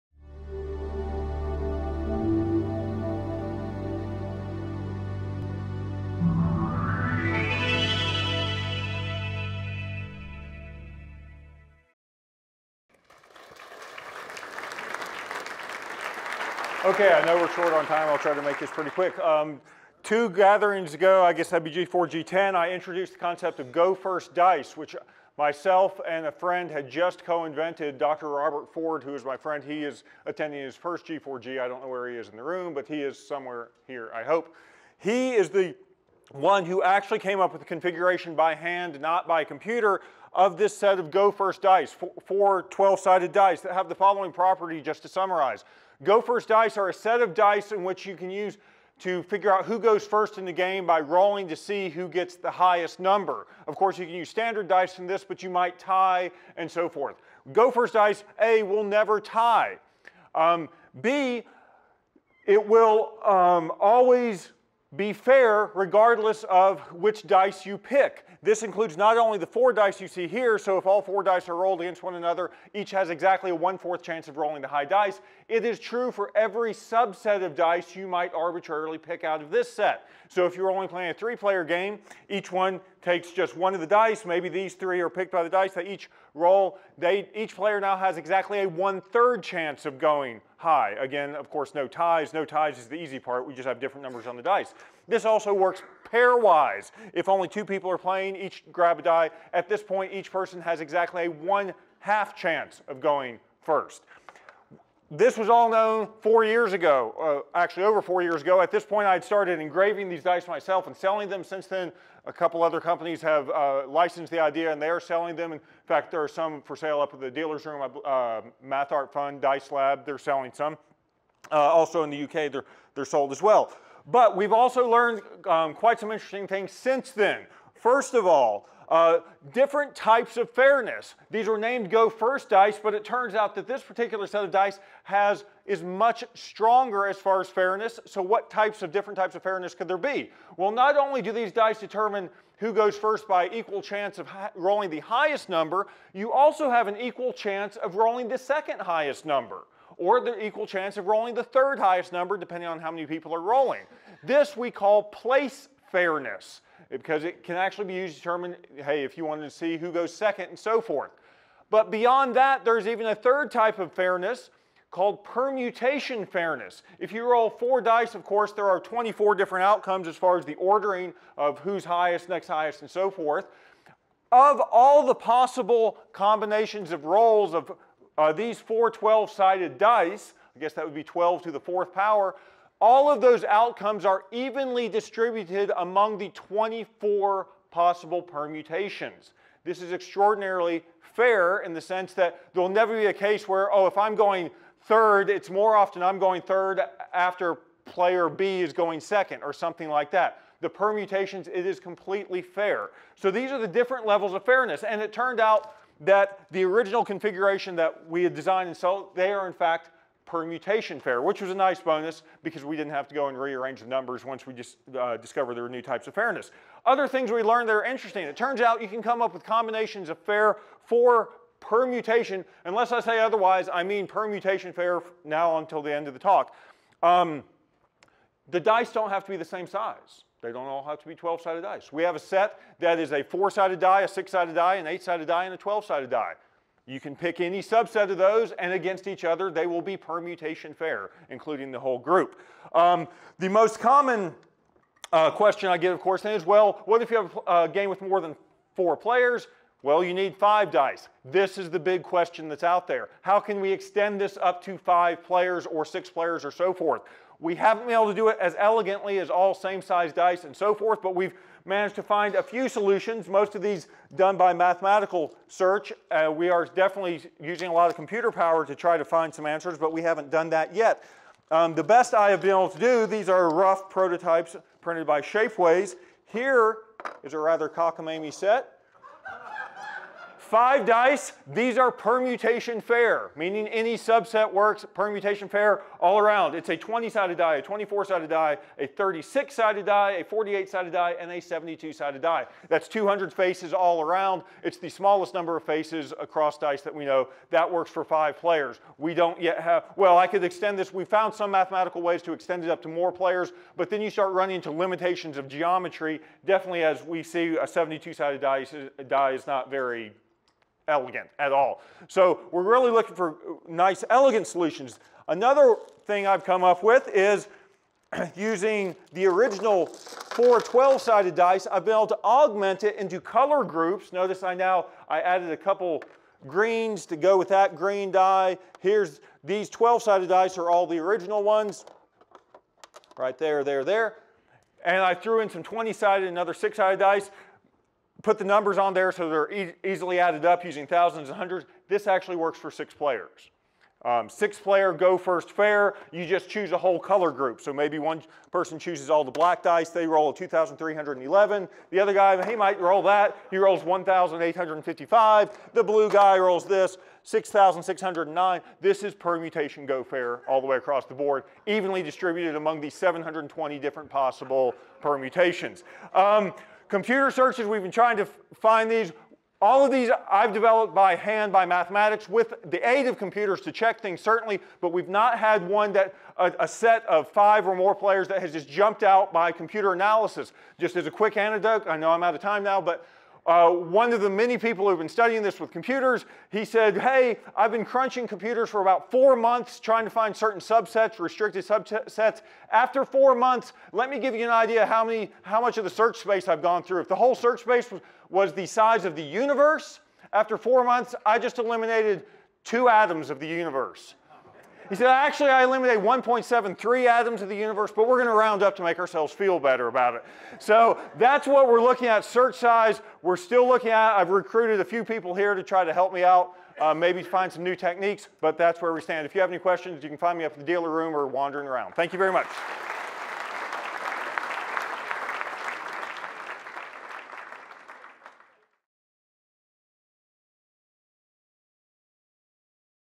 Eric Harshbarger - Advances with "Go First Dice" - G4G12 April 2016
The latest developments in Go First Dice; namely, how close are we to achieving a set of "permutation fair" dice that work for up to 6-players.